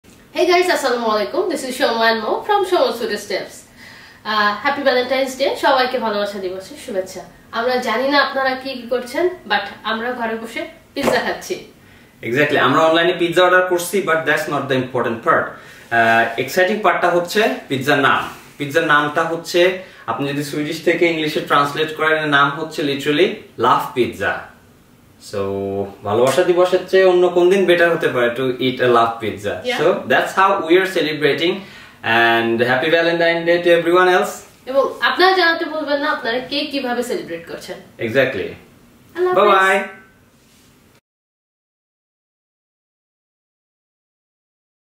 " ভালোবাসা পিৎজা" নামেও পিৎজা হয় ? | Valentine day special | 2022
" ভালোবাসা পিৎজা" নামেও পিৎজা হয় ?

We wanted to order pizza and suddenly we found a pizza on the list called "Kärleks Pizza", If we translate it then it is called LOVE PIZZA. No better day to try this than Valentine Day.

সবাইকে ভালোবাসা দিবসের শুভেচ্ছা 🥰🥰